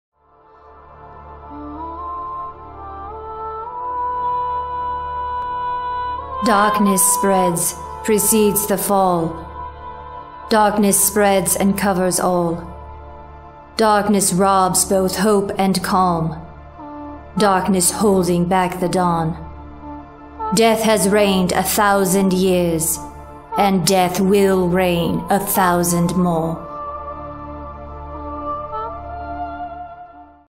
Shows to See in Albuquerque: Days of the Dead (Queen's Prophecy)
Shows to See in Albuquerque: Days of the Dead
See excerpts of elite Dance and Theatre’s productions, which feature our elite Company (Professional and Pre-professional artists) in Ballet, Jazz, Tap, Aerial, Swing, Acting and Vocal.